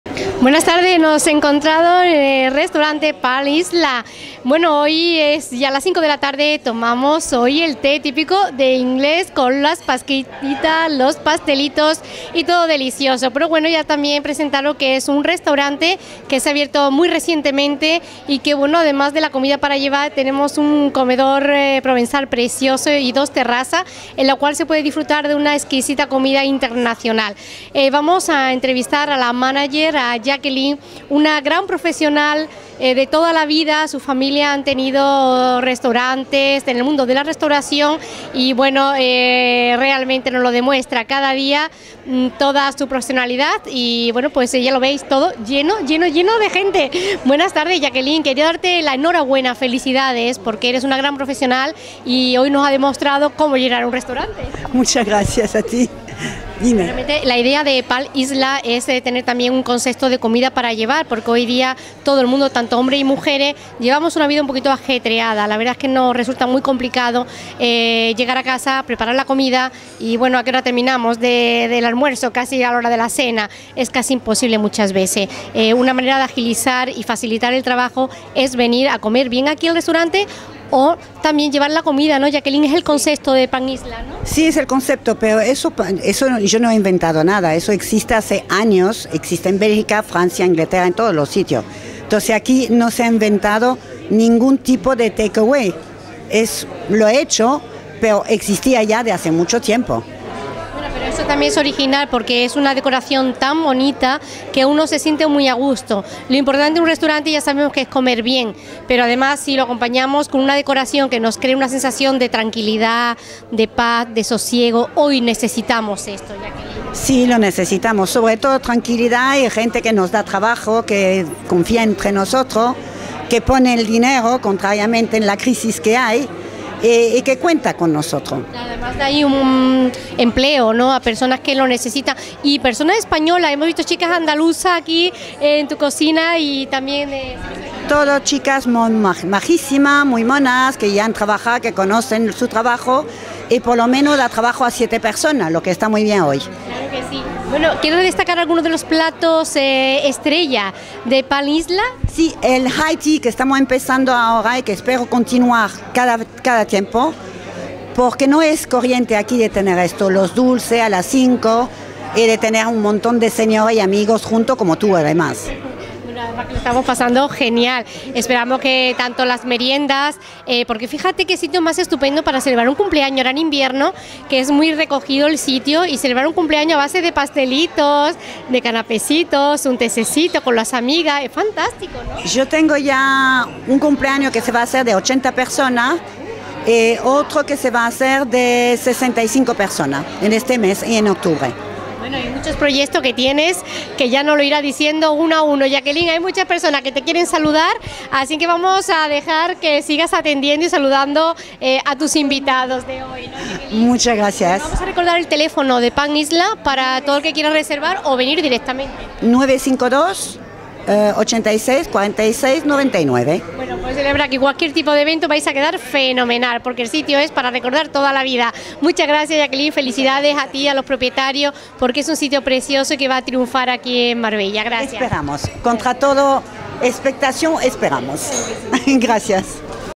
Palm Isle Restaurante, Entrevista de Helena Olaya a Jaqueline Geraerts
Centro Comercial Oasis,Carretera de Cádiz,km 183.Local Nº 11,Milla de Oro. Tf.952.86.46.99